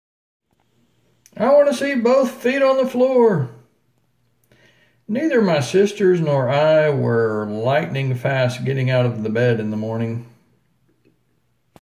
Things I remember my Dad saying #99.
"I want to see both feet on the floor"!

Memories of my Dad:  Dr. Grover D. Barnes,

by his Son:  Dr. Darrell D. Barnes.